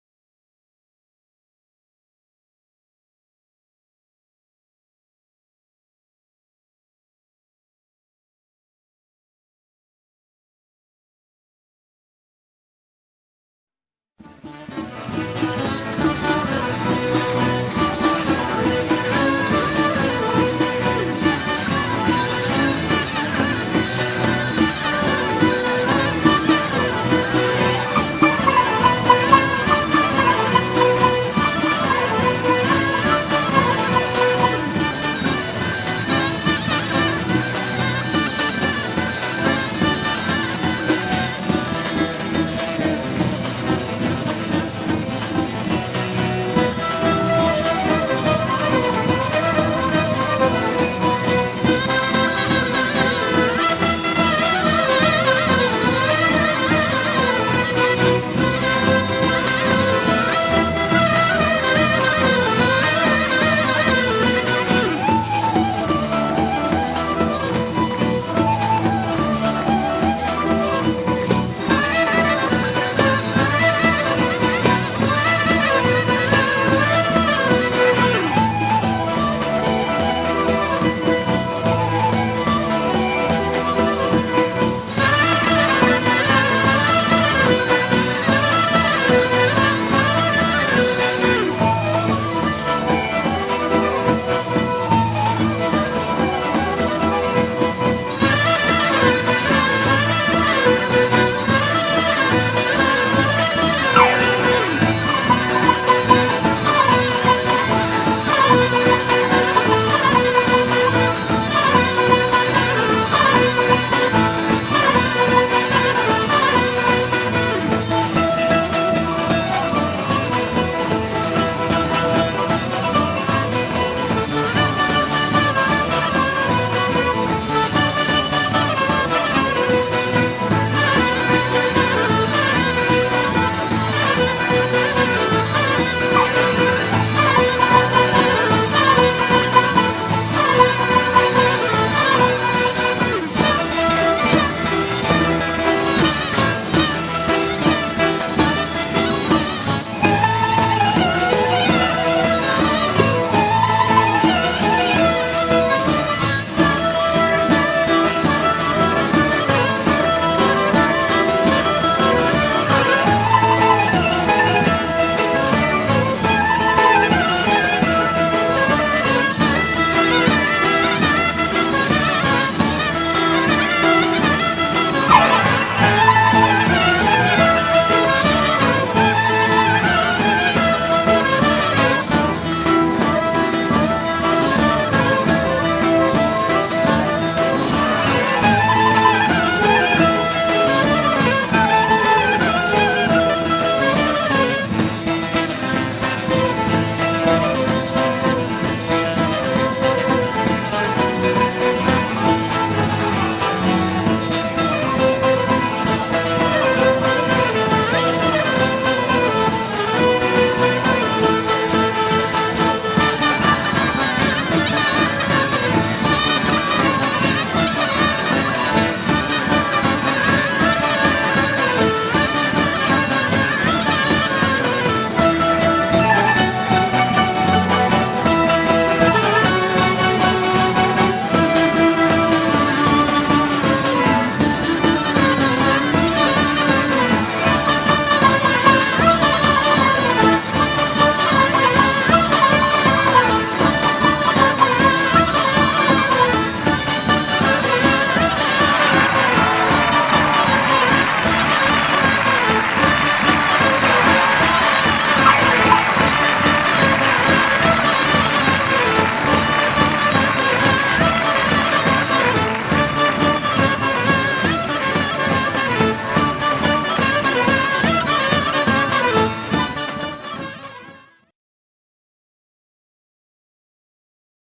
SUMHOTER-2012 (Karadeniz)
Değerli dostlar; Sümer Halk Oyunları Topluluğu-SÜMHOTER'in 38nci kuruluş yıldönümünü kutladığımız RENGARENK ANADOLU adlı Halk oyunları şölenimizin Karadeniz (Kız) ekibi... İyi seyirler dileriz...